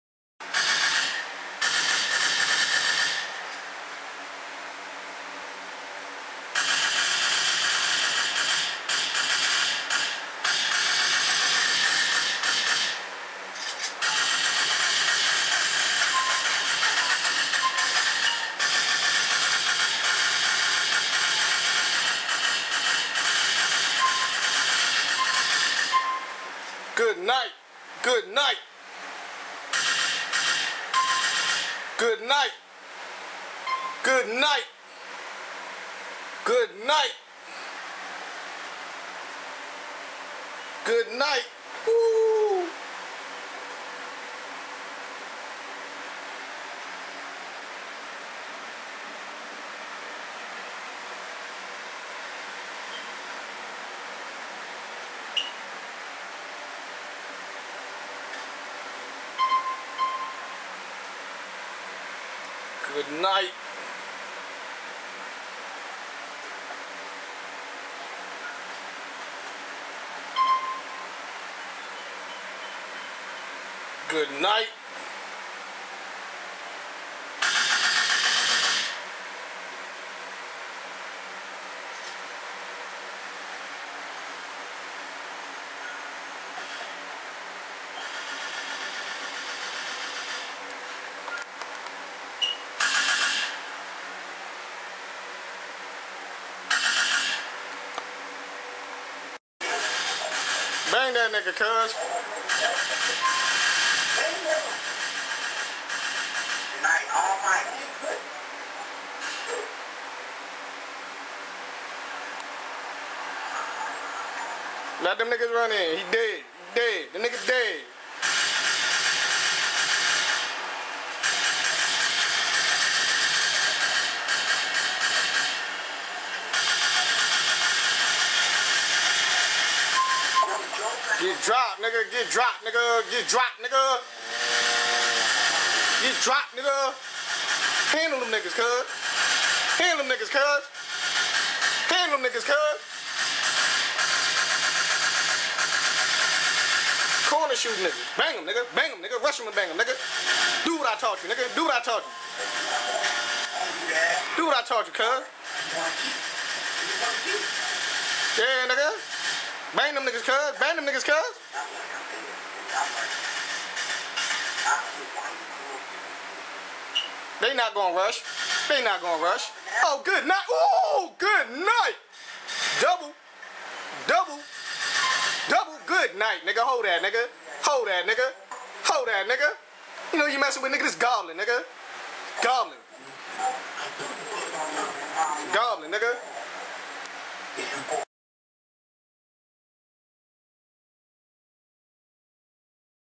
GOBLIN vs HMC3_CLutCH,SquadX-Murine,AlmightyClipz
it waz a fair 3 vs 3 untill at the end they invited alot of spectators to get free kills then LOADED started to kill people to